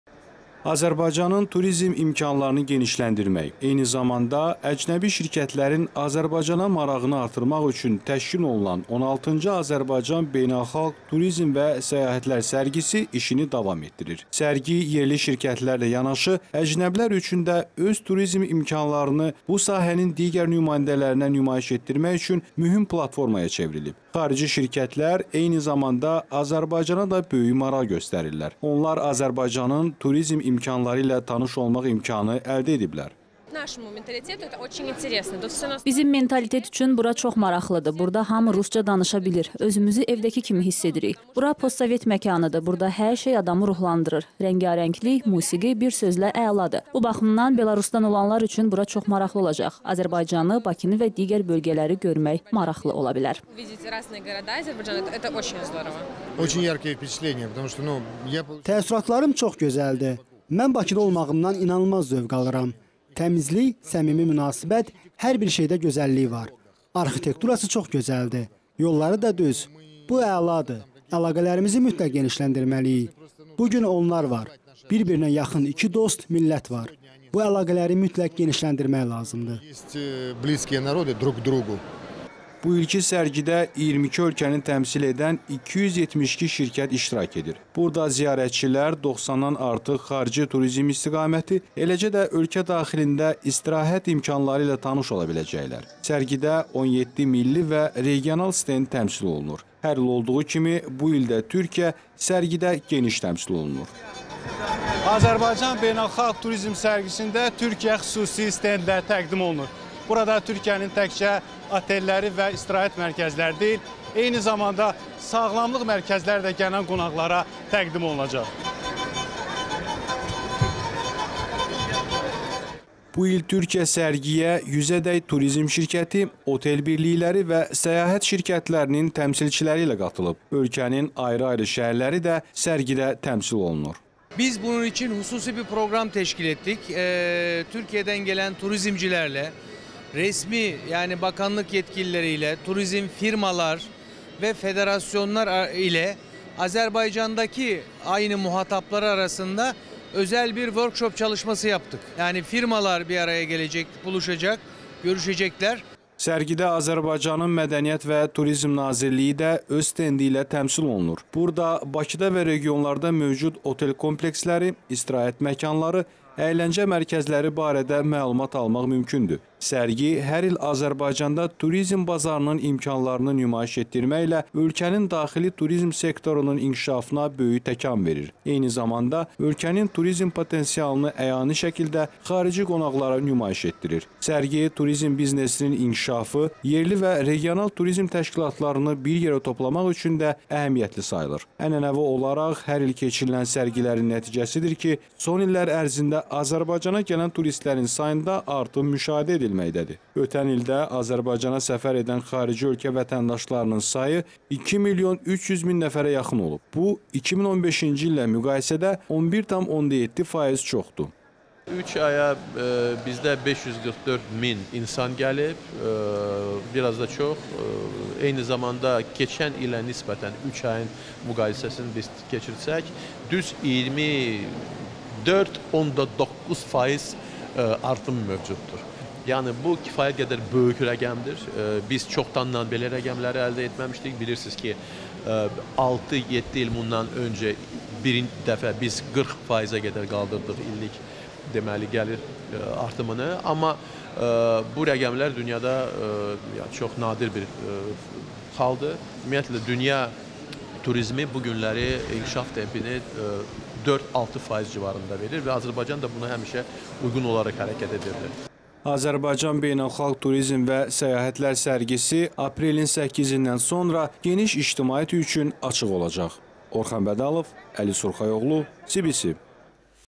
BEYNƏXALQ TURİZM VƏ SƏYAHƏTLƏR SƏRGİSİ DAVAM EDİR
Azərbaycanın turizim imkanlarını artırımaq, eyni zamanda əcnəbi şirkətlərin Azərbaycana marağını artırmaq üçün təşkil olunan 16-cı Azərbaycan Beynəlxalq Turizim və səyahətlər sərgisi işini davam etdirir. Sərgi yerli şirkətlərlə yanaşı, əcnəbilər üçün də öz turizm imkanlarını bu sahənin digər nümayəndələrinə nümayiş etdirmək üçün mühüm platformaya cevirib. Xarici şirkətlər eyni zamanda Azərbaycana da böyük maraq göstərirlər. Onlar Azərbaycanın turizm imkanları ilə tanış olmaq imkanı əldə ediblər.
Builki sərgidə 22 ölkəni təmsil edən 272 şirkət iştirak edir. Burada ziyarətçilər 90-dan artıq xarici turizm istiqaməti, eləcə də ölkə daxilində istirahət imkanları ilə tanış ola biləcəklər. Sərgidə 17 milli və regional stend təmsil olunur. Hər il olduğu kimi bu il də Türkiyə sərgidə geniş təmsil olunur.
Bu il Türkiyə sərgiyə 100-dək  turizim şirkəti, otel birlikləri və səyahət şirkətinin təmçilçiləri qatılıb. Ölkənin   ayrı-ayrı şəhərləri də sərgidə təmsil olunur.
Azərbaycan Beynəlxalq Turizm və Səyahətlər Sərgisi aprelin 8-dən sonra geniş ictimaiyyət üçün açıq olacaq.